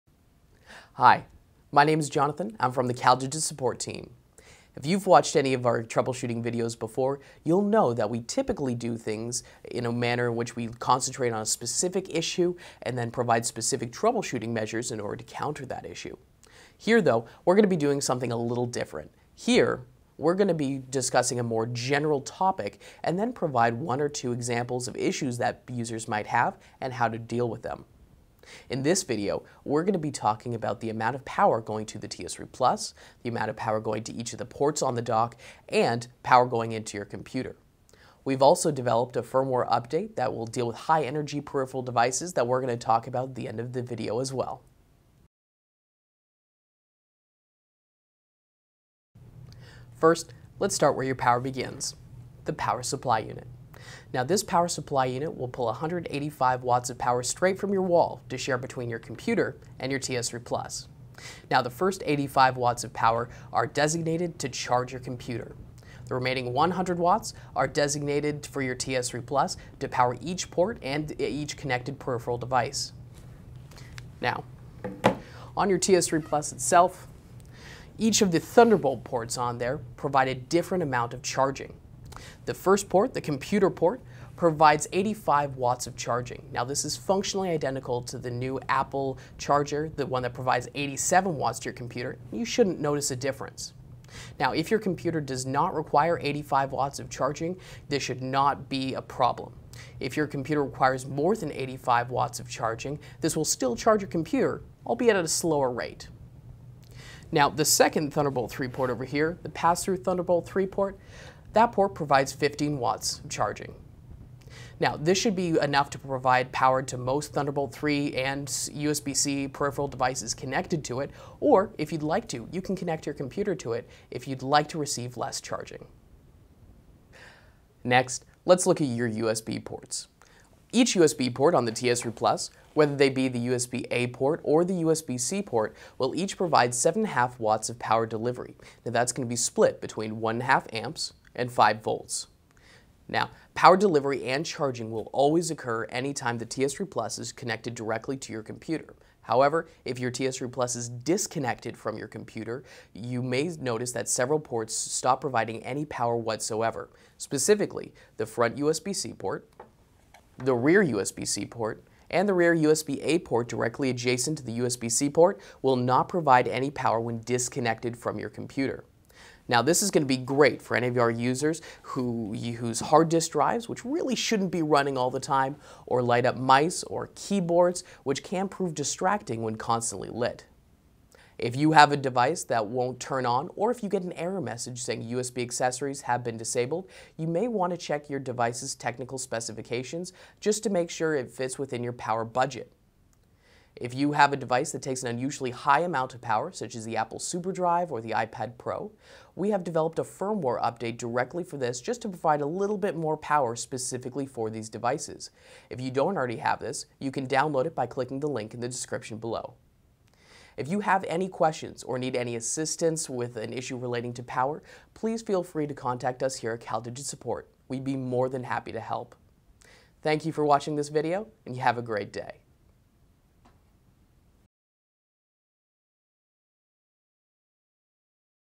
CalDigit Support: TS3 Plus - 85W Charging, USB ports power, Apple SuperDrive Driver
General topics relating to the power of the TS3 Plus:
-Amount of power going to the dock
-Amount of power going to each port of the dock
-Amount of power going into your computer